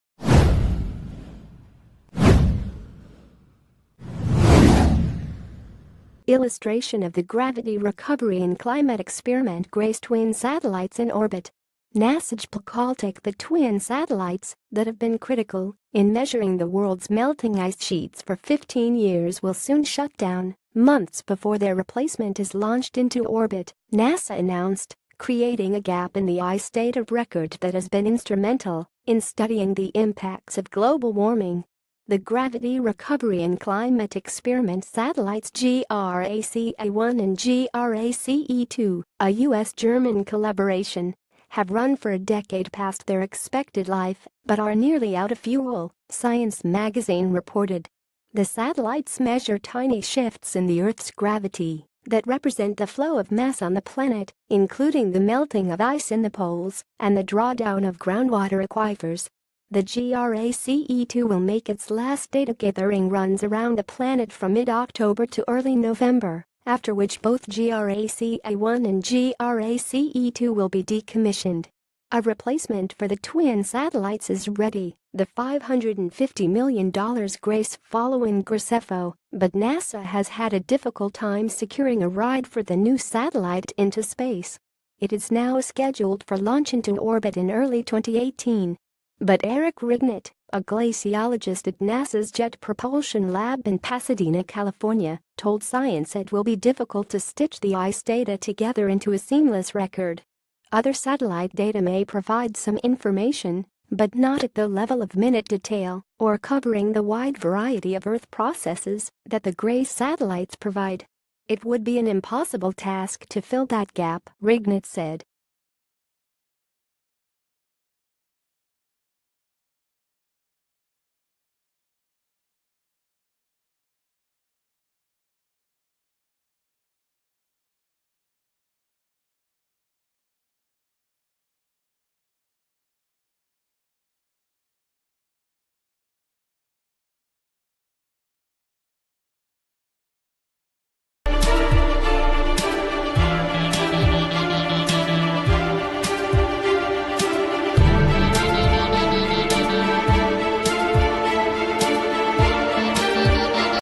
Breaking News | Satellites that measure ice loss to go dark
Satellites that measure ice loss to go dark

Illustration of the Gravity Recovery and Climate Experiment GRACE twin satellites in orbit. NASAJPLC...